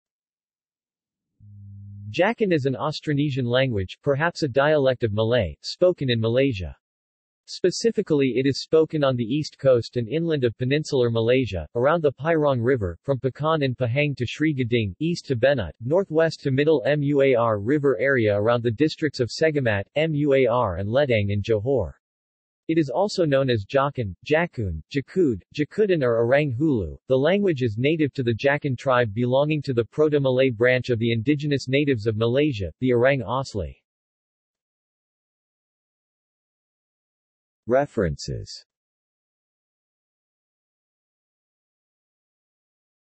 Jakun language | Wikipedia audio article
This is an audio version of the Wikipedia Article:
Jakun language

SUMMARY
Jakun is an Austronesian language, perhaps a dialect of Malay, spoken in Malaysia. Specifically it is spoken on the east coast and inland of Peninsular Malaysia, around the Pairang River, from Pekan in Pahang to Sri Gading, east to Benut, northwest to middle Muar river area around the districts of Segamat, Muar and Ledang in Johor. It is also known as Djakun, Jakoon, Jaku’d, Jakud’n or Orang Hulu.The language is native to the Jakun tribe belonging to the Proto-Malay branch of the indigenous natives of Malaysia, the Orang Asli.